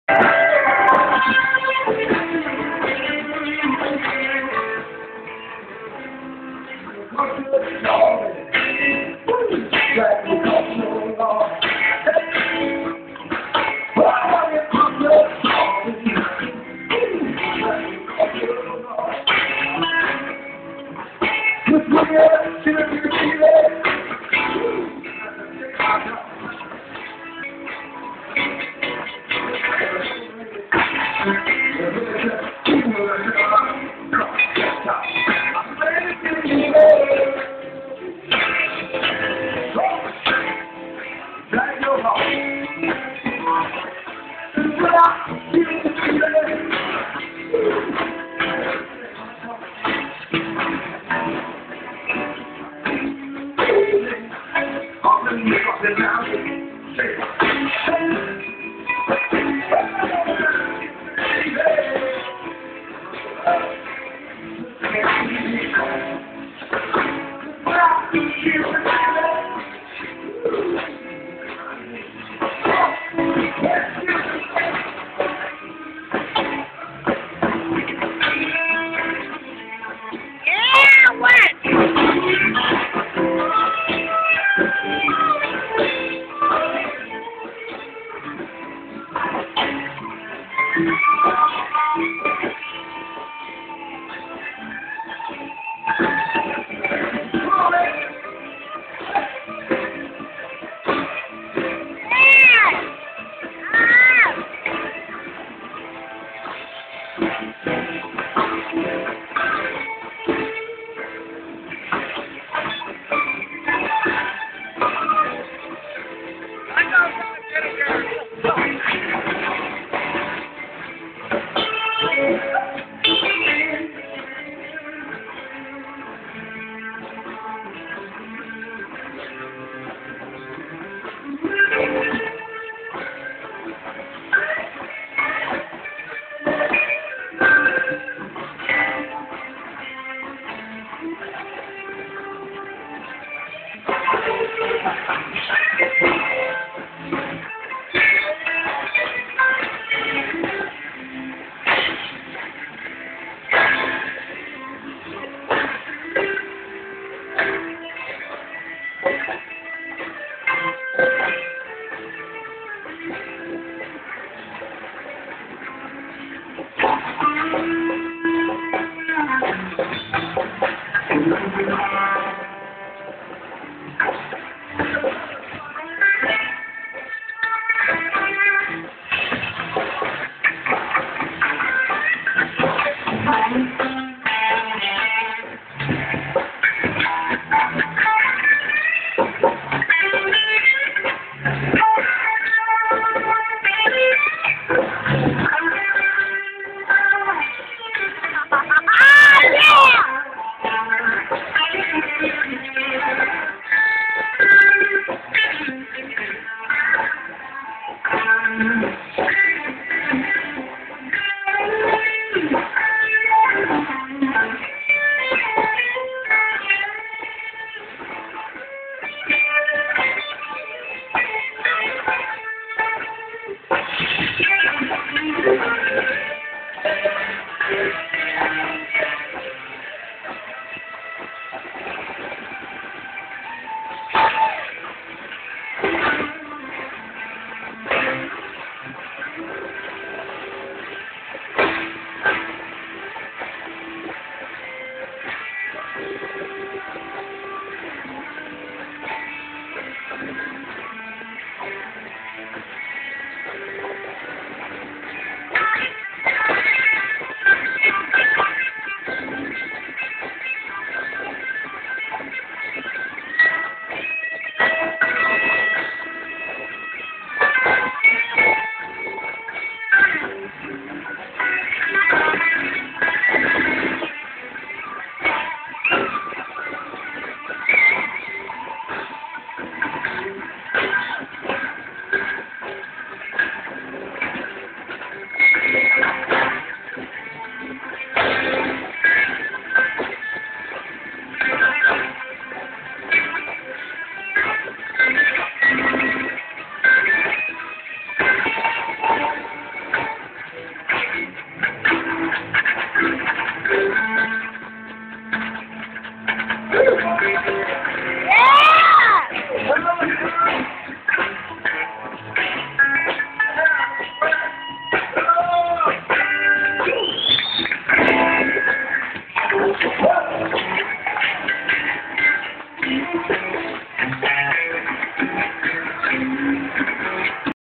The Mike Vohsen Blues Band (2/2)
Wes Dodson, siting in with them nice kids from The Mike Vohsen Blues Band.

Ray's Bar & Grill, Plantersville, Texas

16 JAN 2010